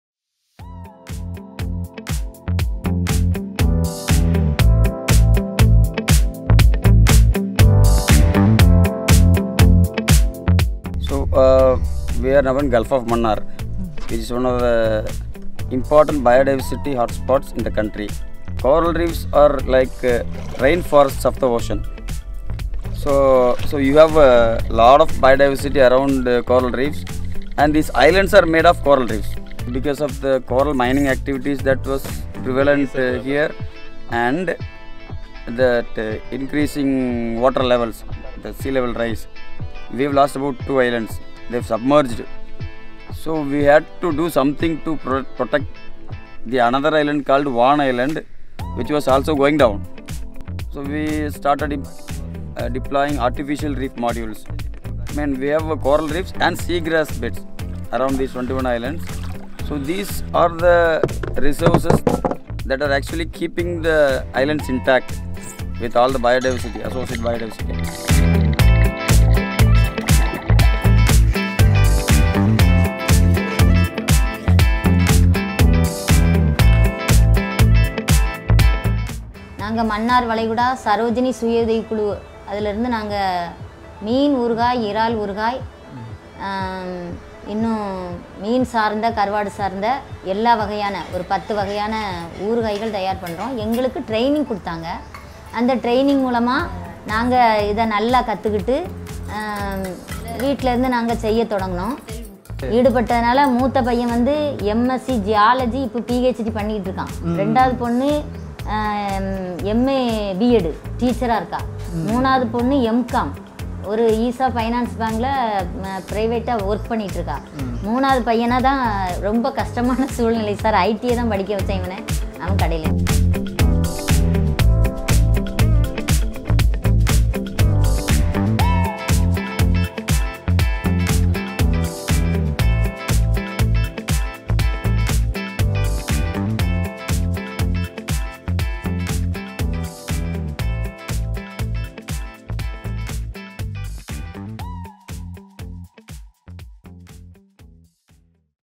Coral Lifebouys of Gulf of Mannar
An initiative funded by the Ministry of Environment, Forest and Climate Change (MoEF&CC) under the National Adaptation Fund for Climate Change (NAFCC), through NABARD, is restoring fragile marine ecosystems and strengthening coastal livelihoods.​

The project focuses on reviving coral reefs and seagrass beds and installing artificial reef structures—steps that have led to richer marine biodiversity and improved fish stocks.​

Watch the video to see the inspiring journey.​